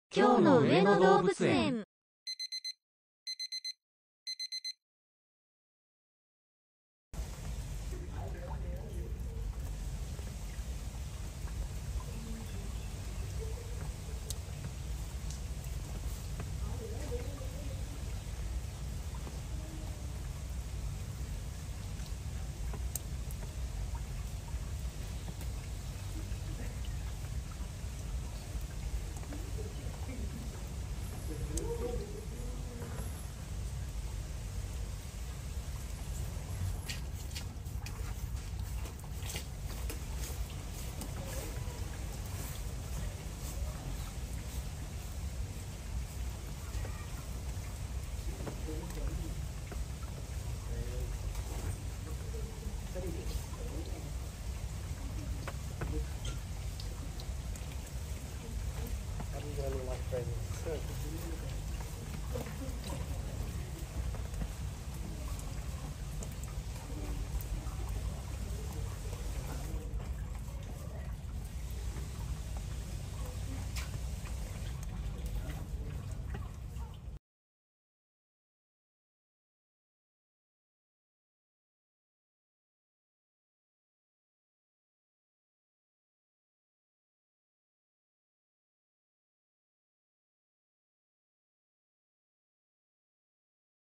ドアを叩いてお部屋に帰りたいポンくん【‎2024年‎10月8日】【上野動物園】エゾヒグマ
エゾヒグマの様子です【‎2024年‎10月8日】【上野動物園】